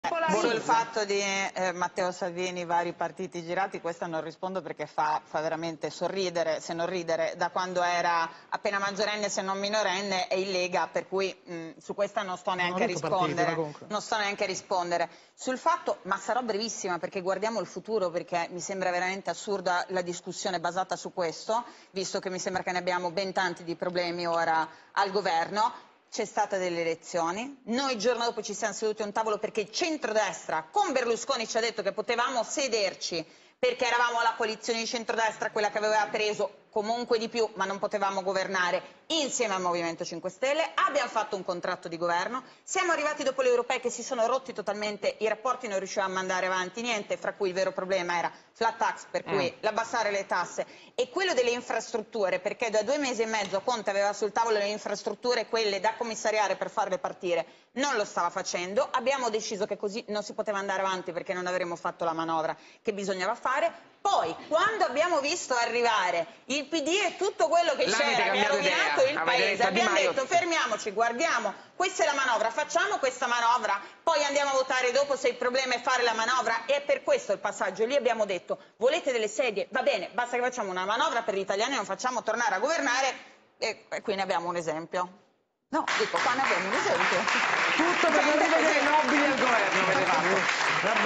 Lucia Borgonzoni, Lega: "Berlusconi ci disse che potevamo sederci al tavolo coi 5 Stelle per un ...
Lucia Borgonzoni (Lega): "Abbiamo detto ai 5 Stelle 'Volete delle sedie?' Ok, basta che facciamo prima una manovra per gli italiani e poi torniamo a votare, ma non facciamo tornare a governare... qui ne abbiamo un esempio (indicando Luciano Nobili)".